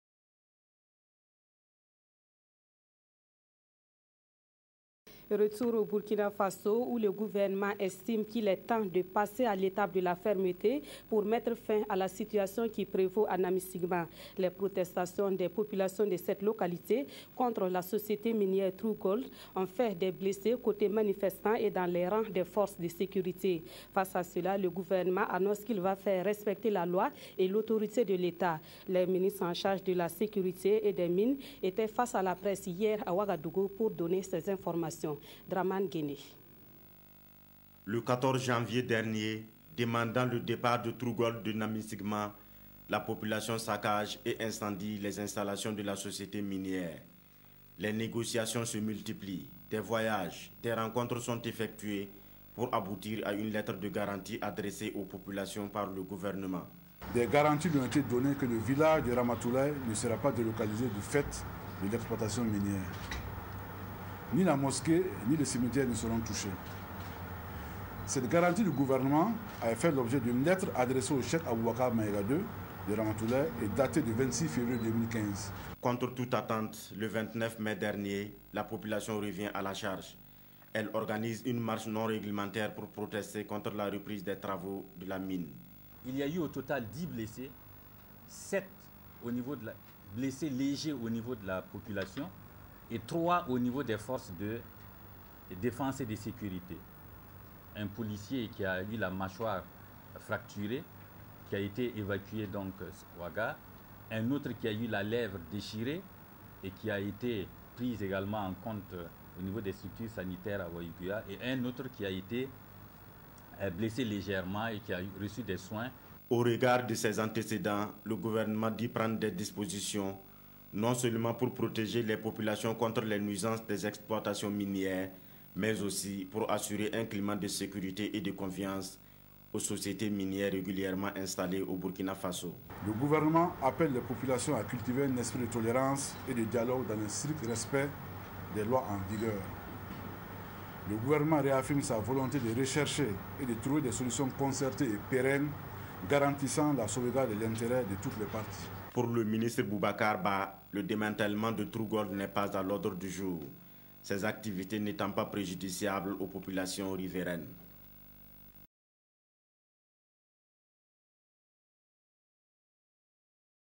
RTB/Economie-Le ministre en charge de la sécurité était face a la presse
Le ministre en charge de la sécurité était face a la presse pour parler des mesures prises par le gouvernement sur la situation a Namissiguima